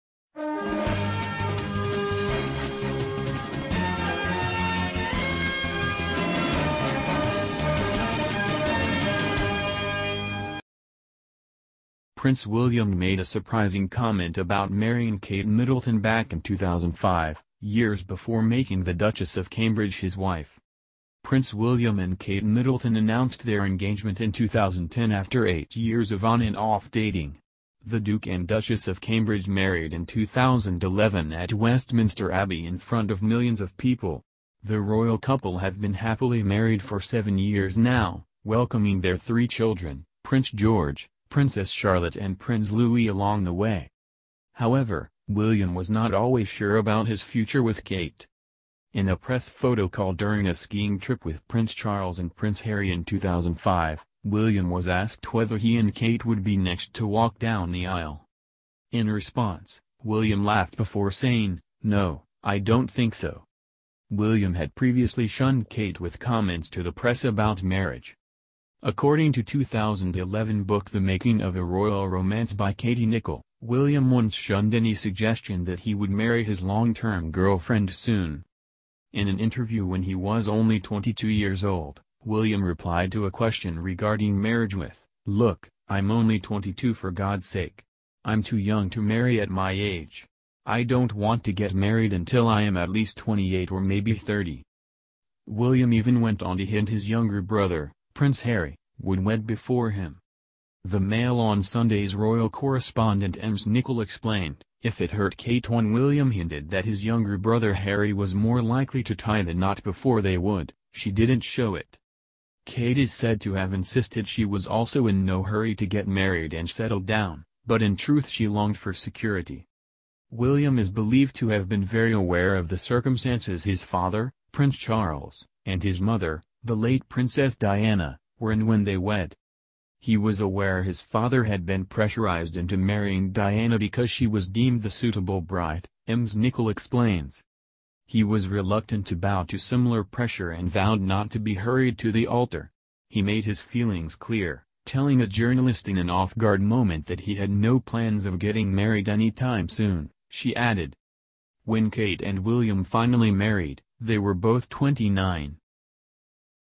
Royal SNUB: Prince William’s surprise answer to Kate Middleton marriage question revealed
PRINCE William made a surprising comment about marrying Kate Middleton back in 2005, years before making the Duchess of Cambridge his wife.